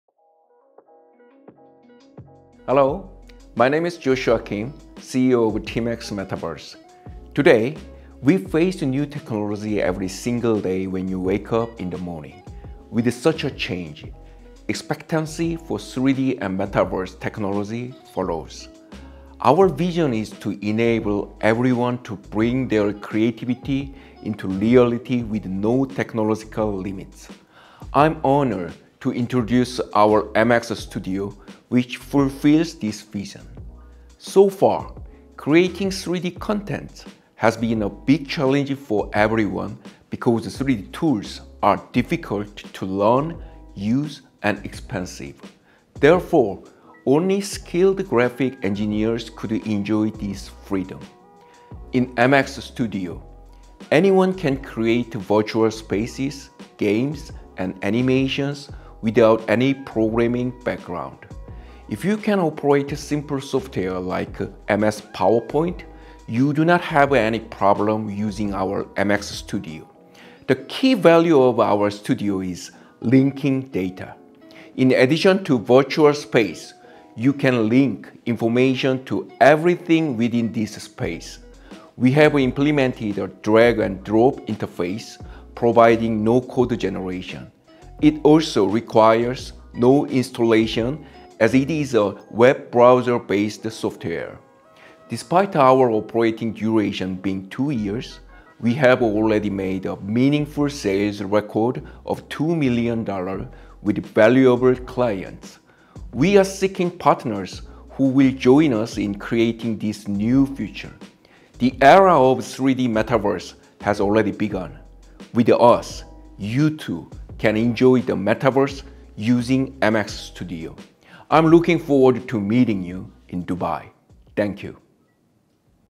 [TmaxMetaverse] MX Studio, Where your creativity comes to life
MX Studio is a all-in-one tool to express your creativity

In-browser, no-code, real-time rendering. Reinventing the way 3D content is created and distributed. Build your limitless creativity.

● Drag & Drop
● Cross-Platform Compatibility
● Add Interaction without Coding
● Support multi-user access